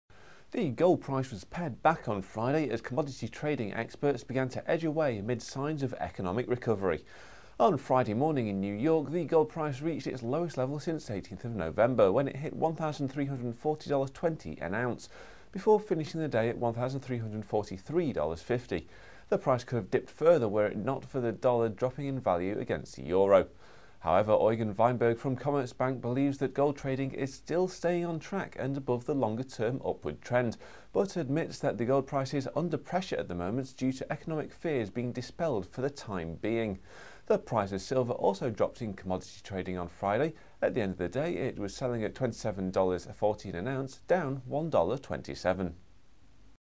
Stronger economy dents gold price
Gold price dips again as economy strengthens